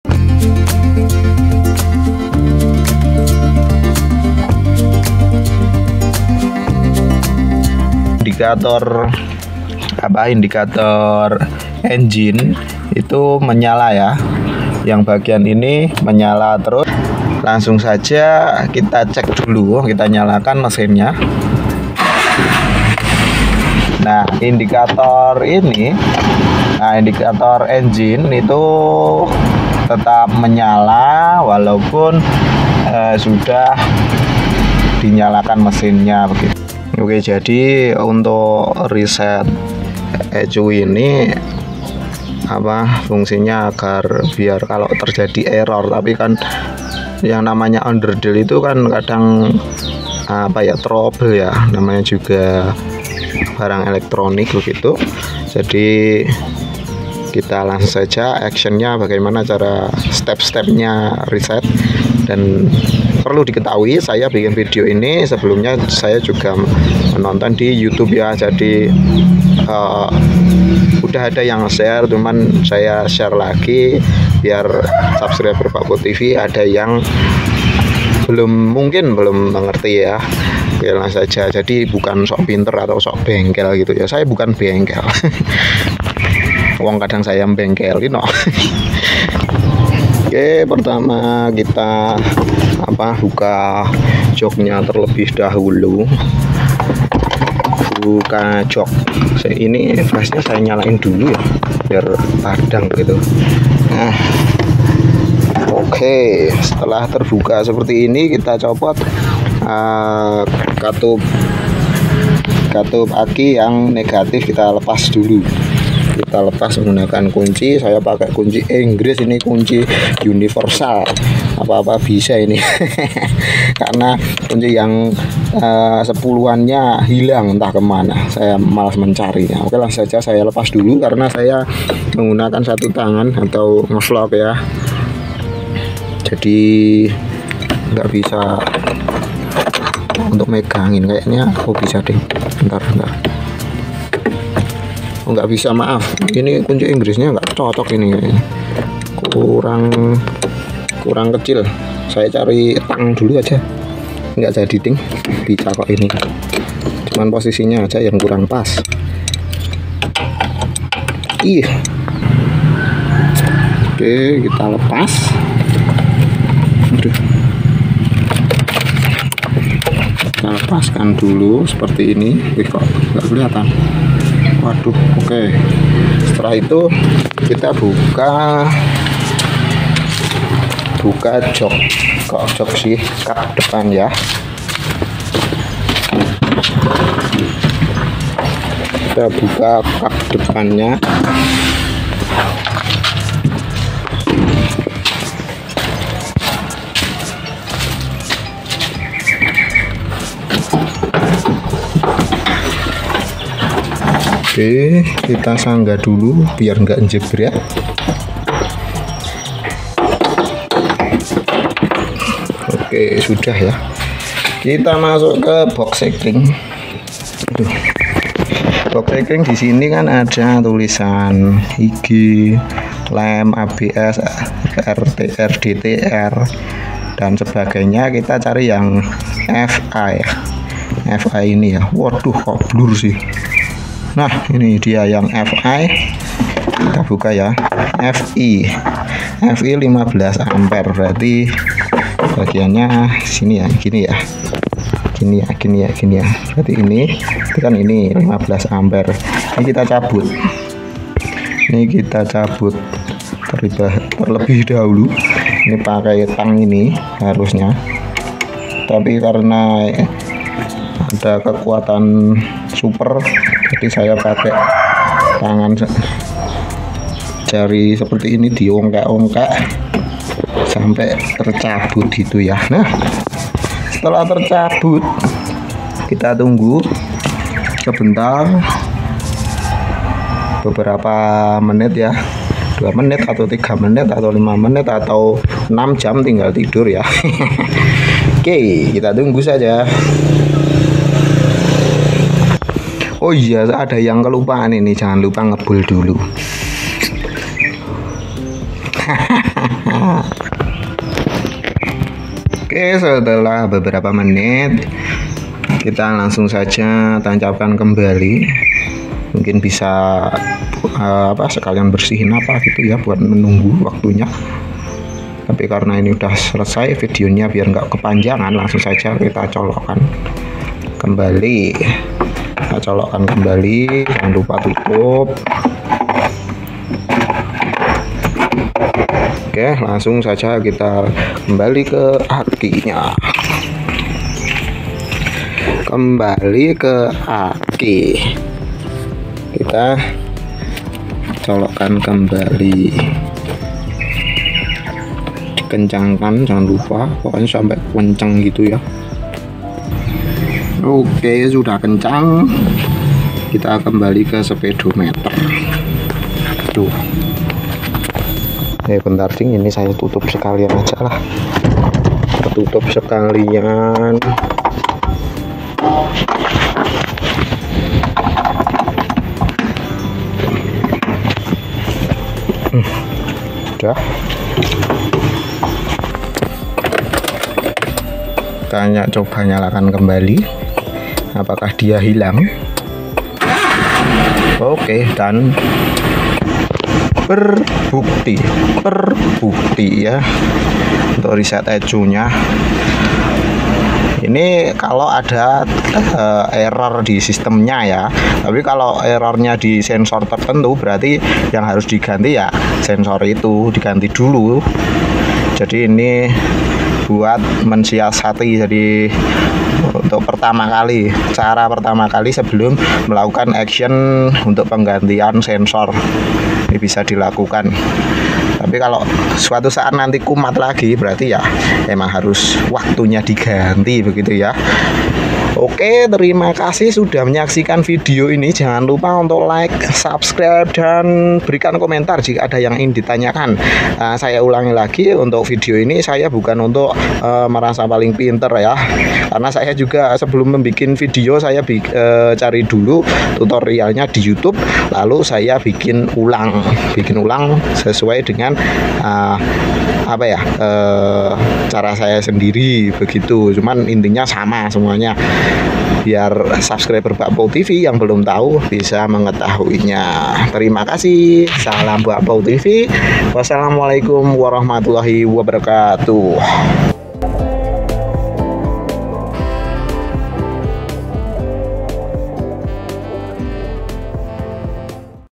Tutorial Reset ECU Suzuki APV Manual Tanpa Scanner OBD2
Cara Reset ECU Suzuki APV Manual Tanpa Scanner OBD2. Caranya gampang banget lho gan. Silahkan di coba!